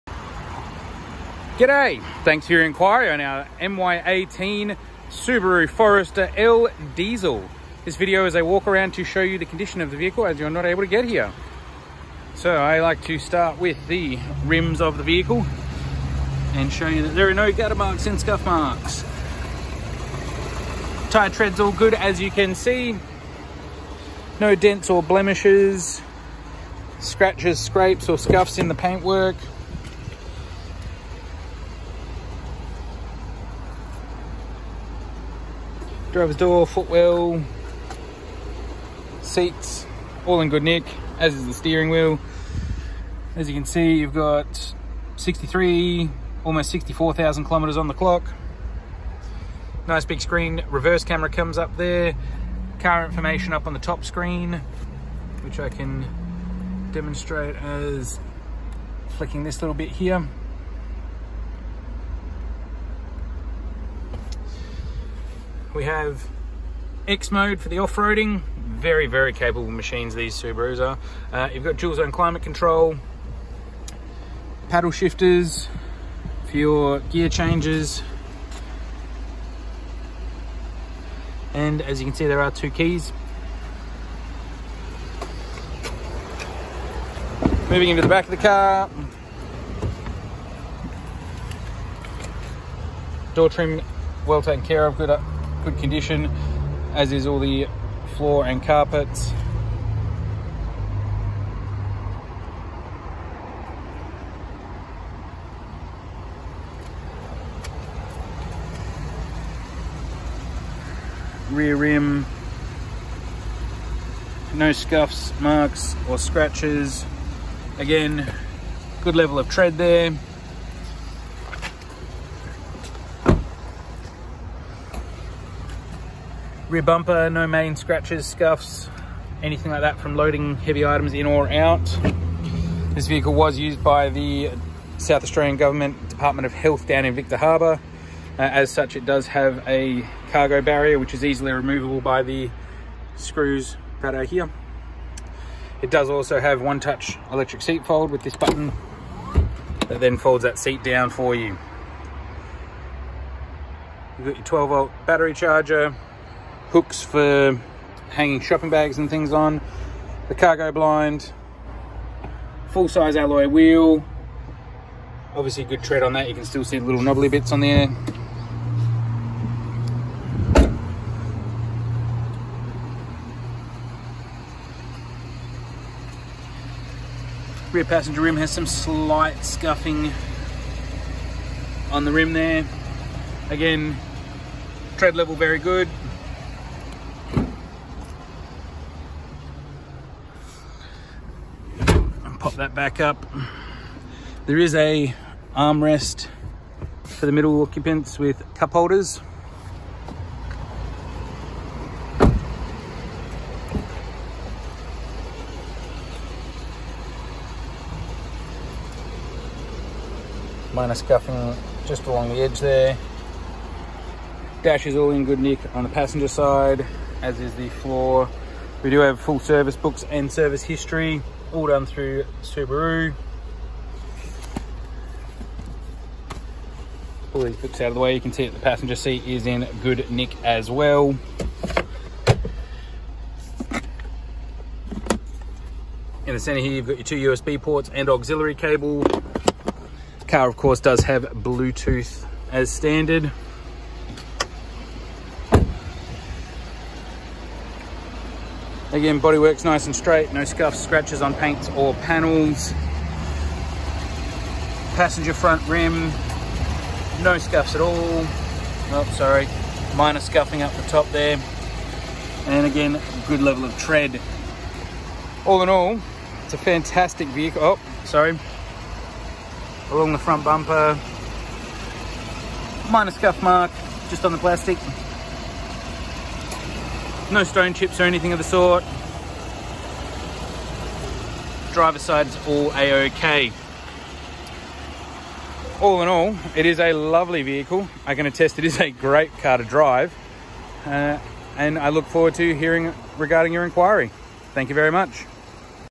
2018 Forester L diesel
Walk around video of Subaru 2018 MY18 Forester L diesel